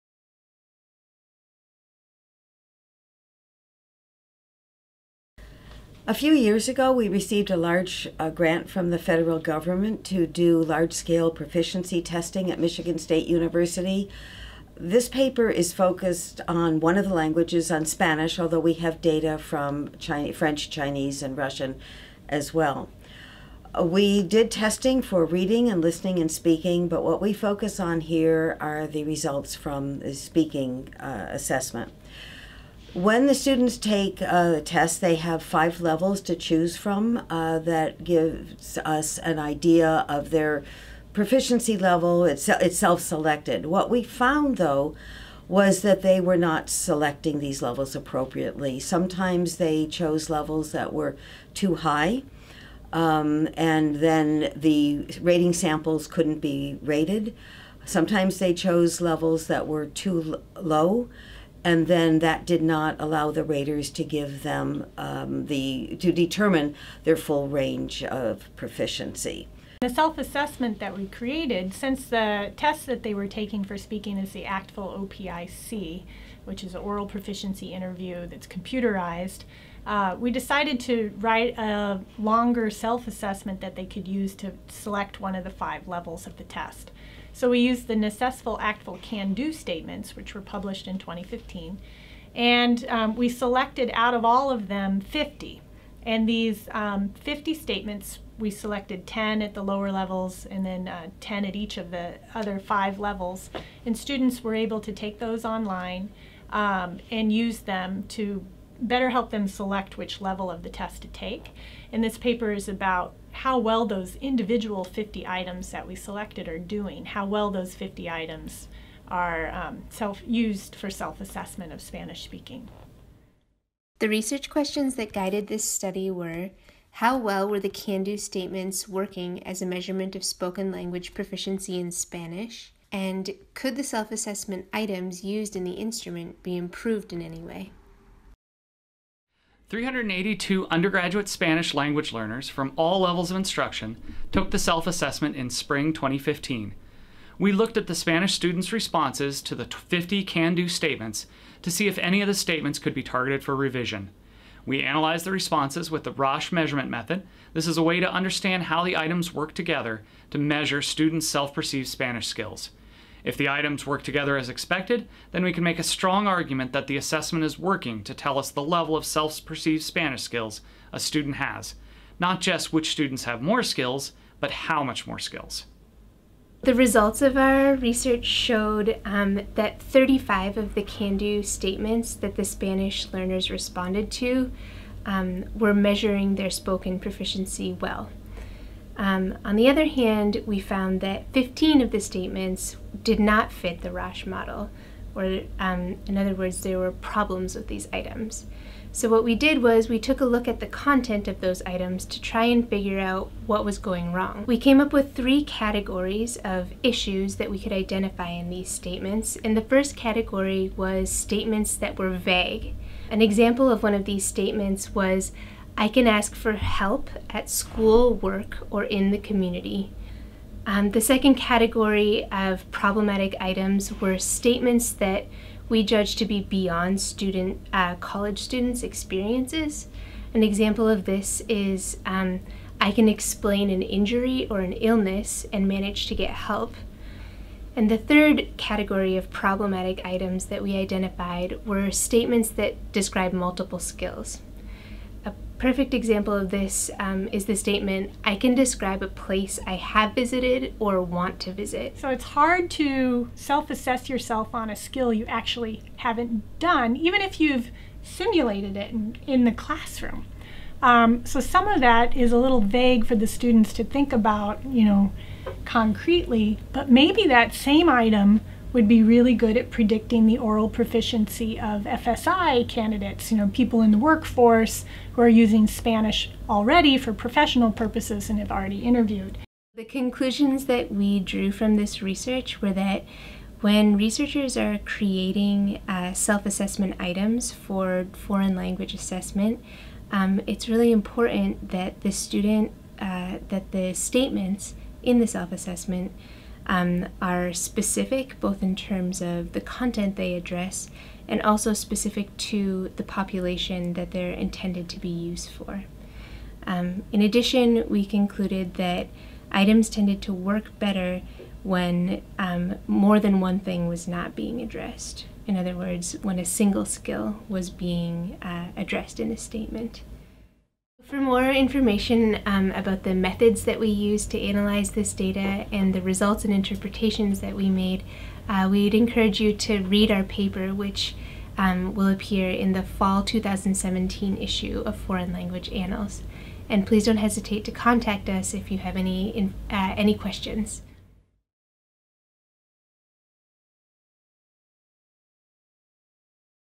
Assessing the Validity of ACTFL Can Do Statments
Tigchelaar, M., Bowles, R. P., Winke, P., & Gass, S. (2017). Assessing the validity of ACTFL can-do statements for spoken proficiency: A Rasch analysis. Foreign Language Annals 50(3).